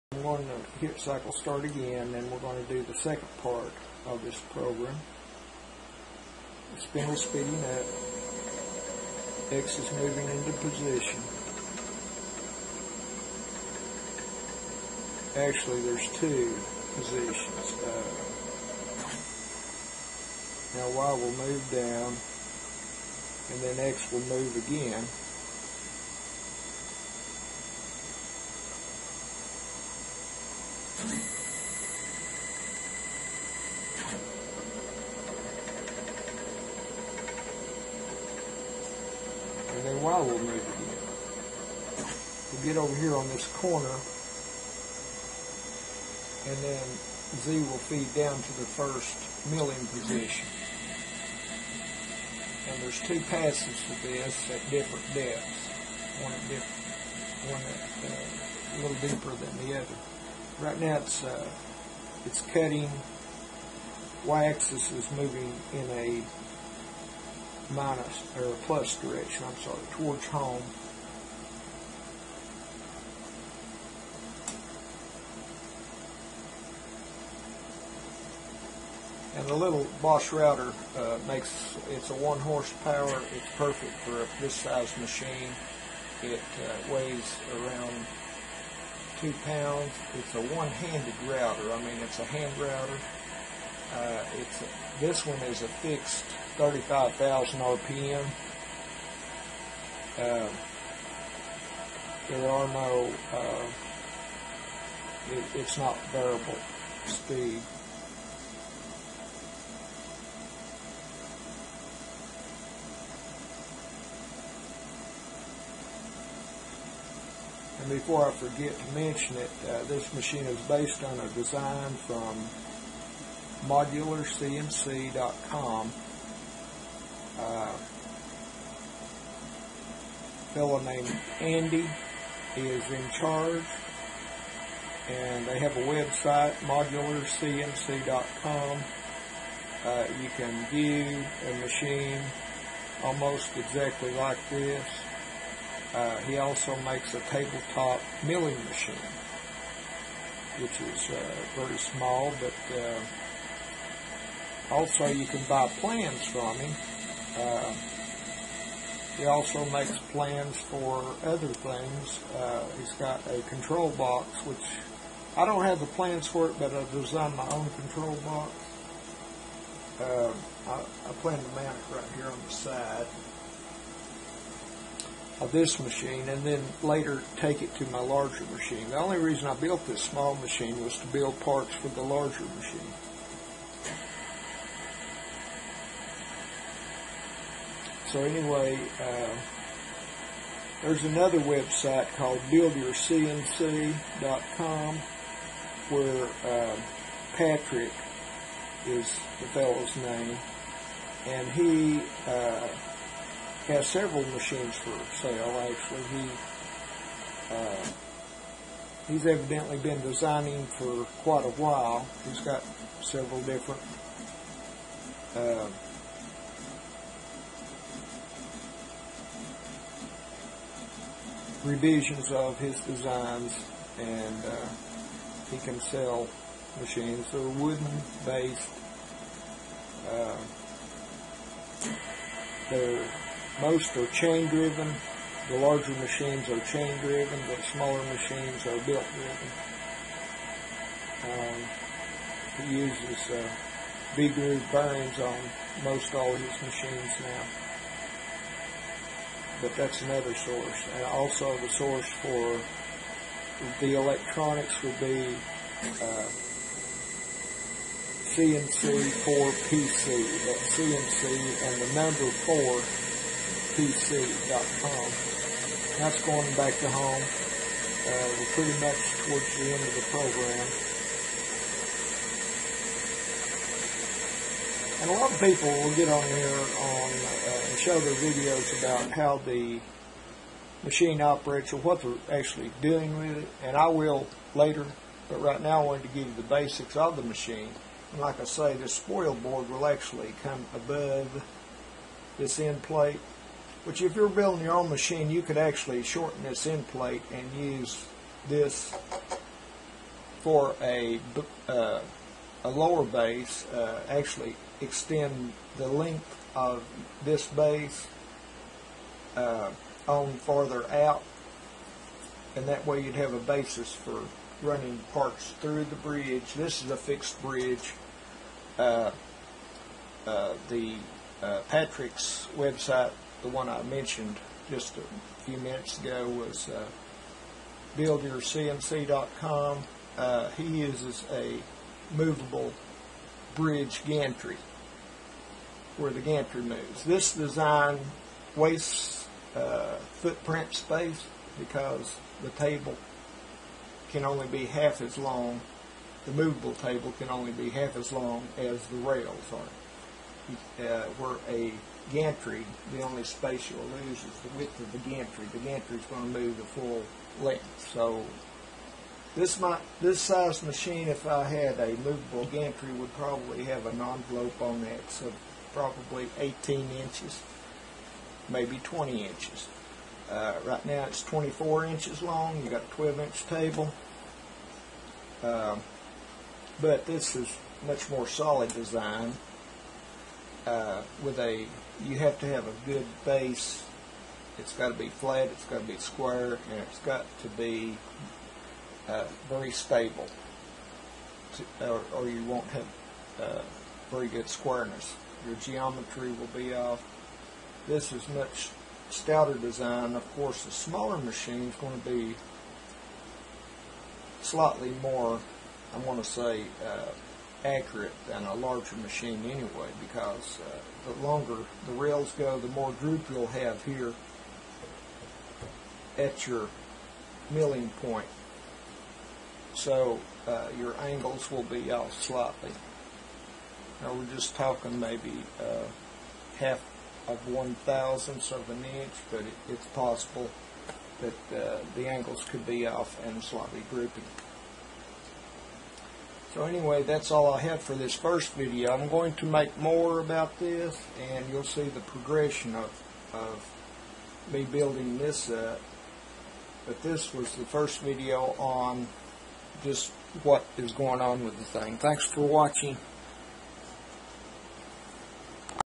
Small CNC Router Intro Part 2.mp4
Building a router for small business or personal use.